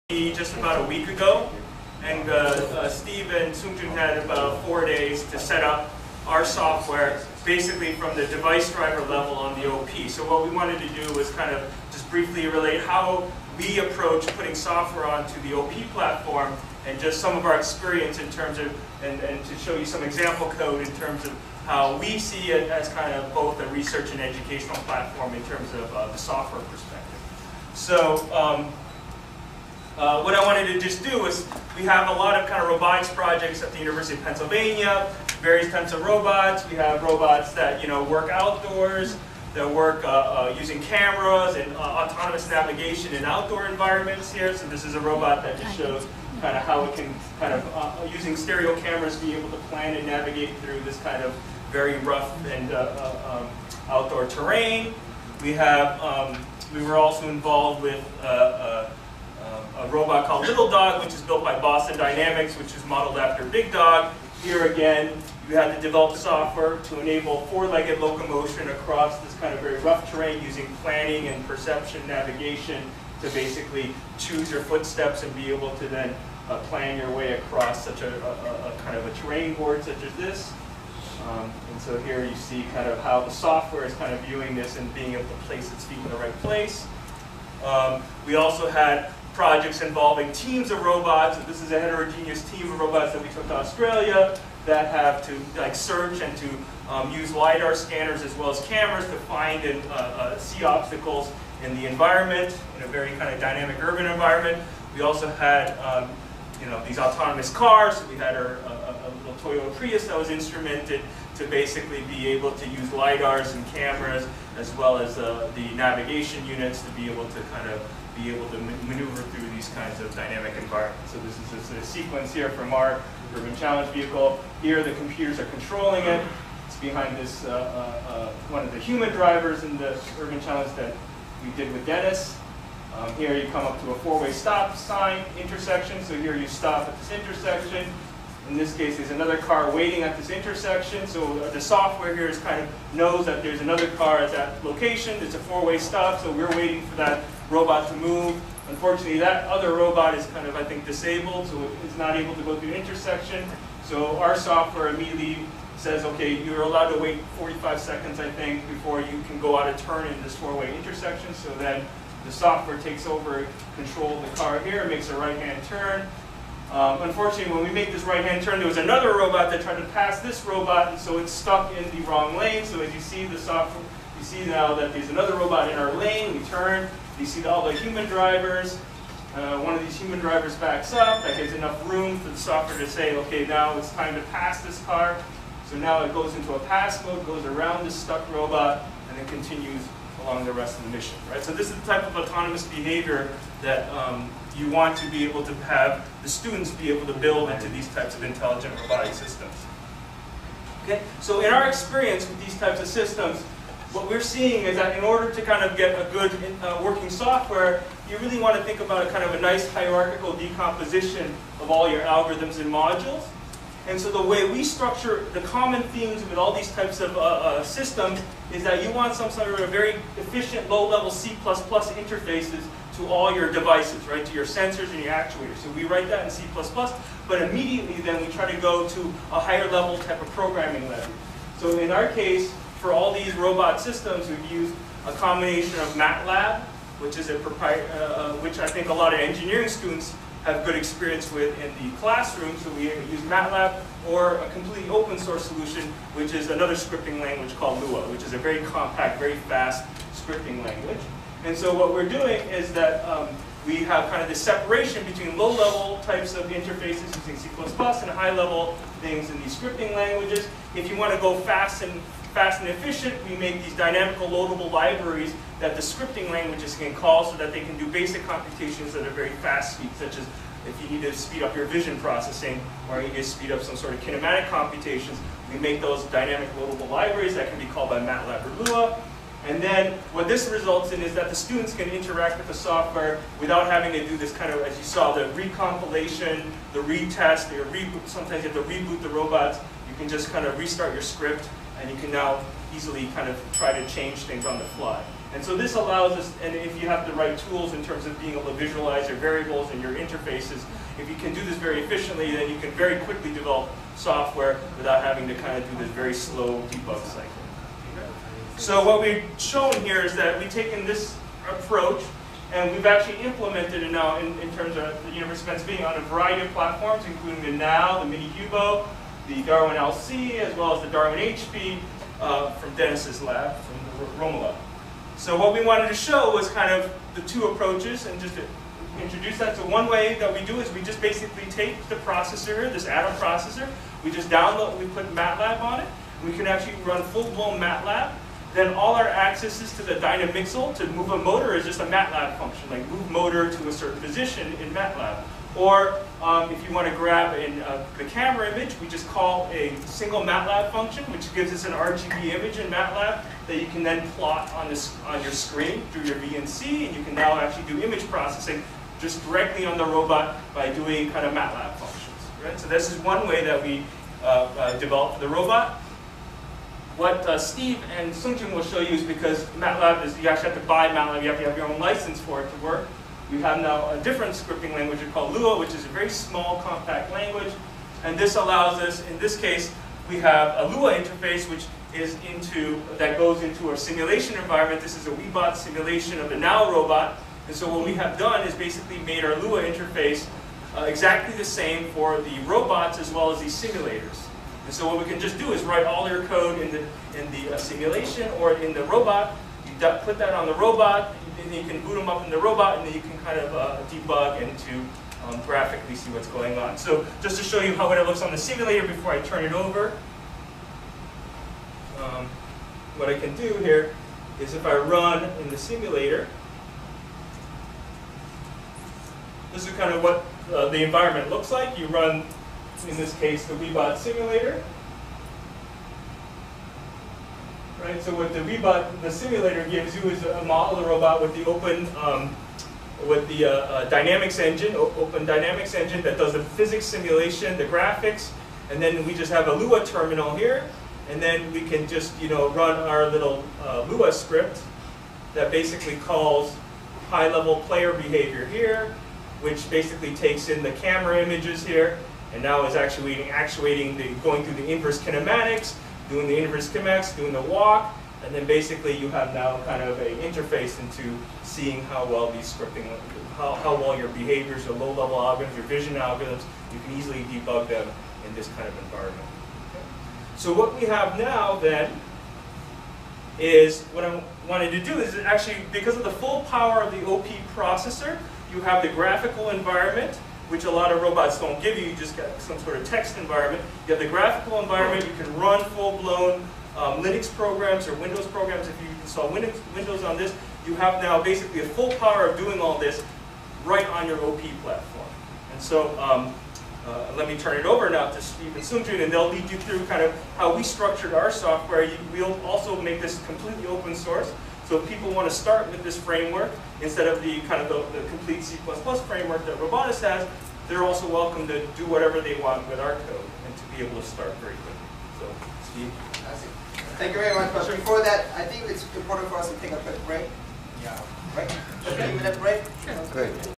ROBOTIS Humanoid robot, DARwIn-OP - Presentation by Dr. Daniel Lee (UPenn)
Open platform humanoid robot, DARwIn-OP